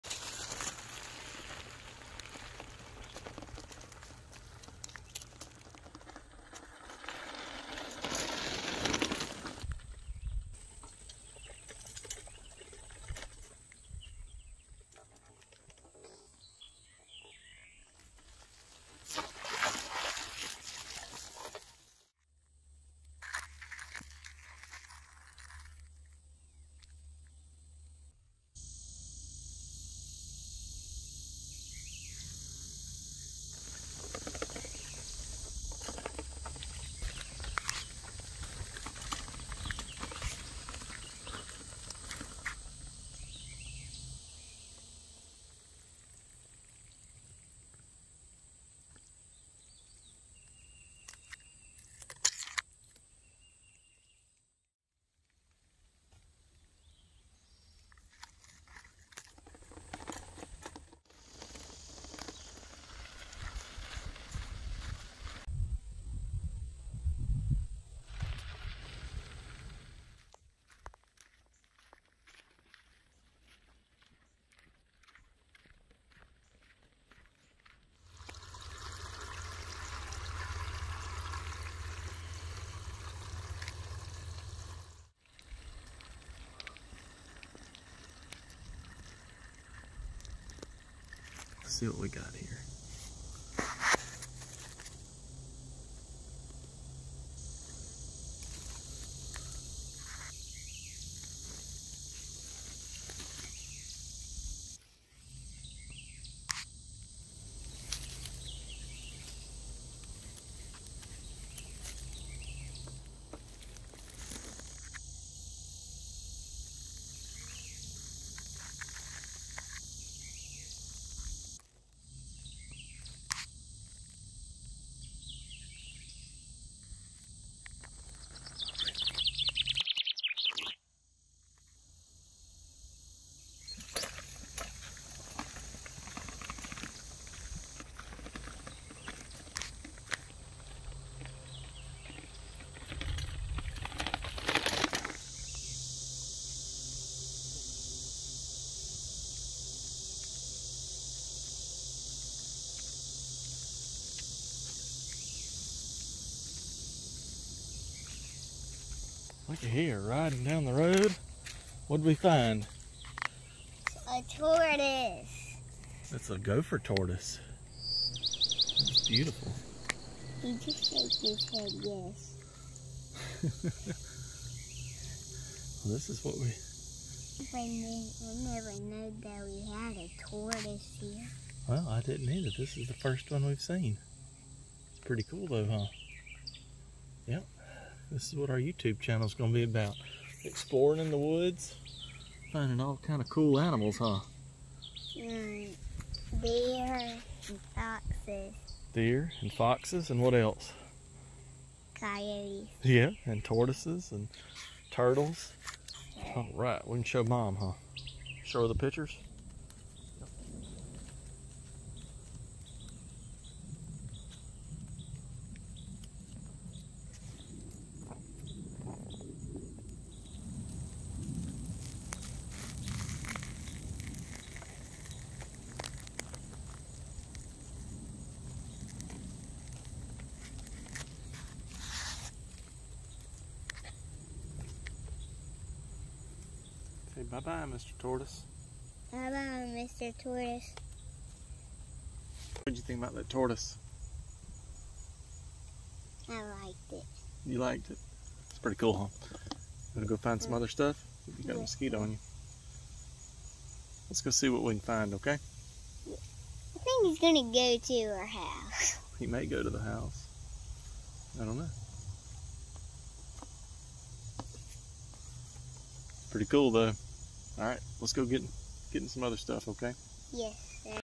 Exploring our property(Did NOT expect to find this!)
Our 1st video. We spent the day exploring our trails around the property and made a cool discovery.